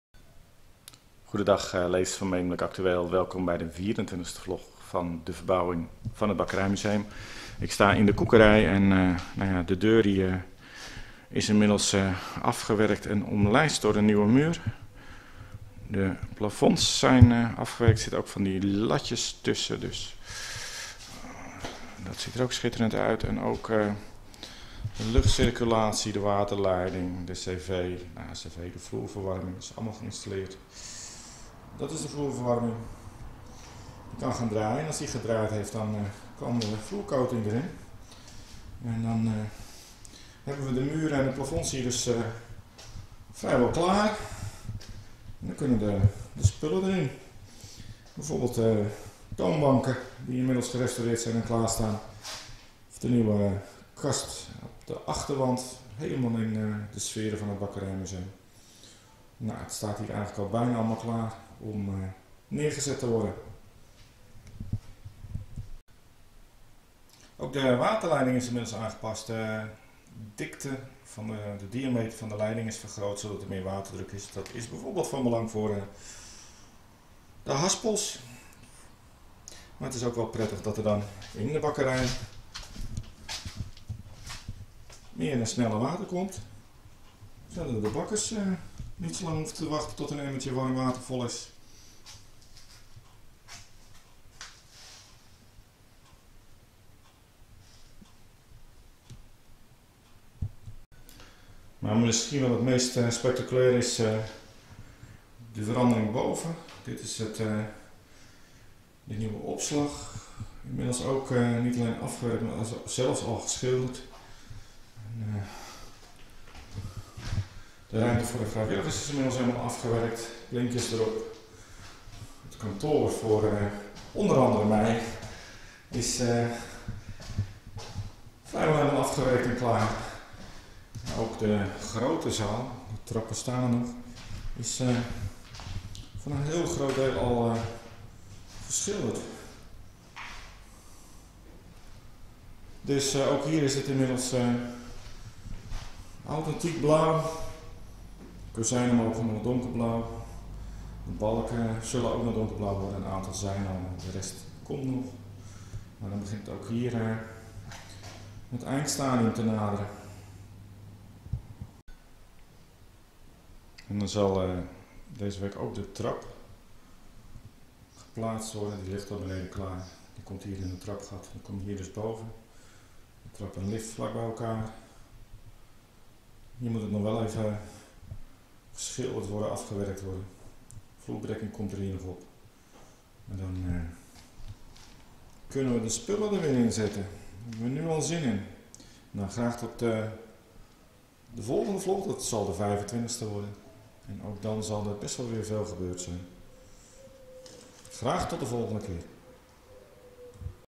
Verbouwing Bakkerijmuseum Medemblik, het einde is in zicht
MEDEMBLIK - In de 24ste vlog is duidelijk te zien dat er steeds meer af komt. In het vernieuwde gedeelte beneden zijn de plafonds en wanden vrijwel klaar terwijl op de bovenverdieping inmiddels ook al grote delen klaar zijn.